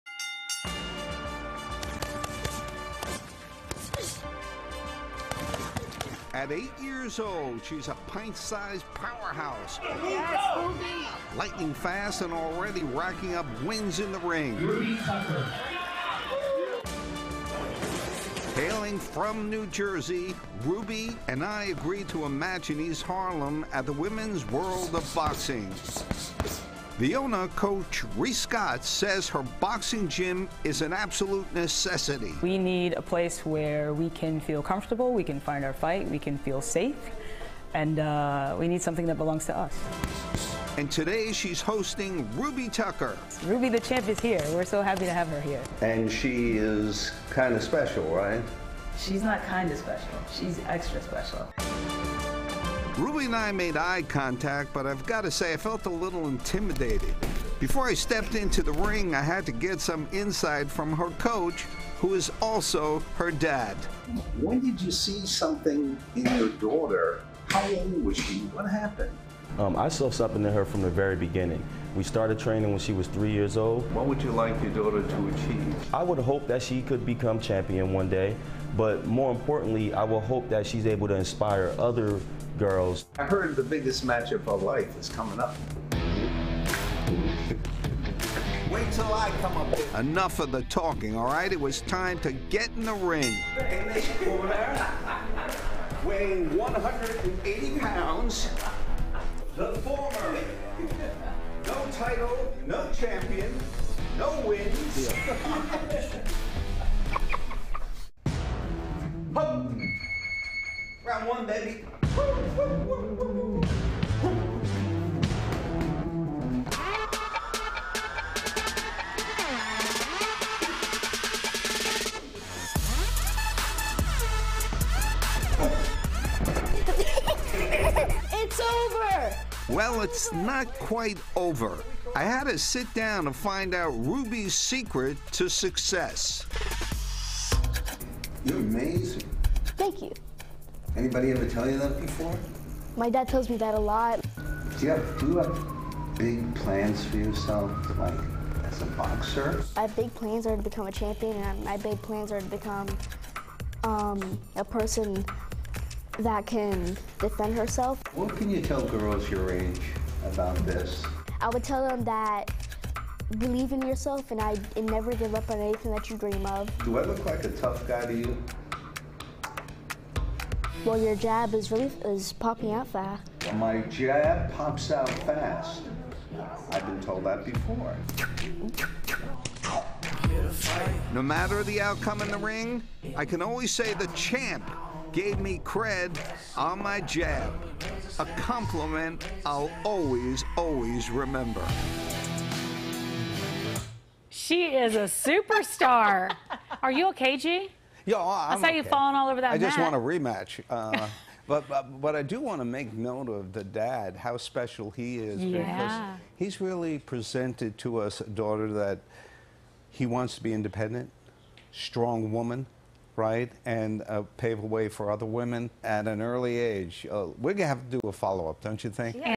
8-year-old boxer making a name for herself in the ring
One New Jersey girl is making headlines in and out of the ring, with her quick footwork and mighty jabs. Mr. G got a chance to get into the ring with Ruby Tucker at the Women's World of Boxing in East Harlem.